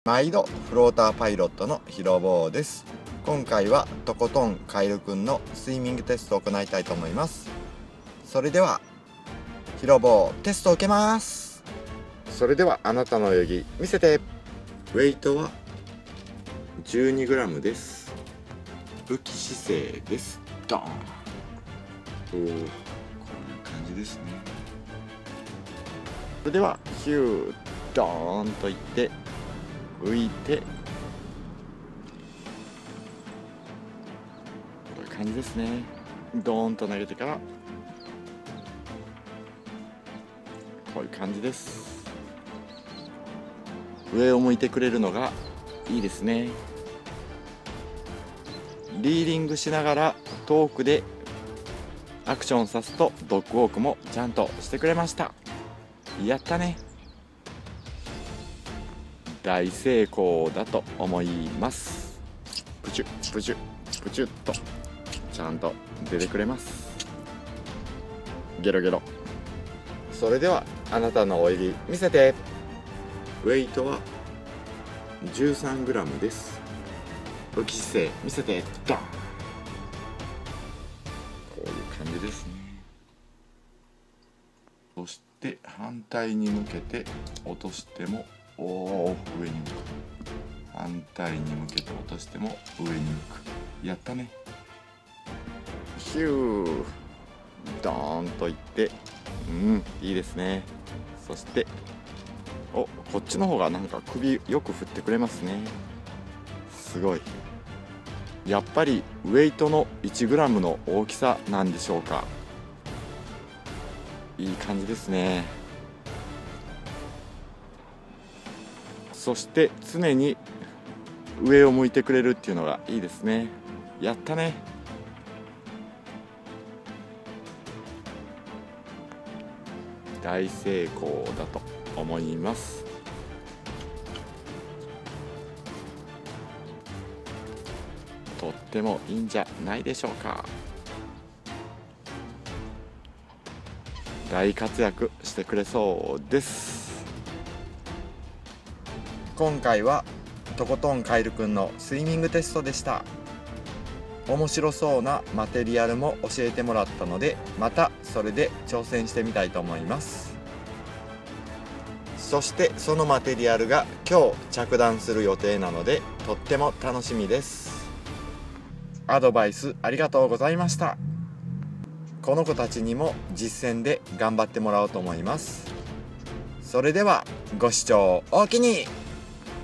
中空フロッグ自作③スイミングテストトップウォータールアー自作|自作ルアー|Frog Lures|lure making|釣りひろ坊Hirobou Fishing
毎度、自作ルアーのひろ坊です♪今回はハンドメイドルアーの中空フロッグルアーの作成動画です♪
少しでもお役に立てれると嬉しいです(#^___^#)

iMovieから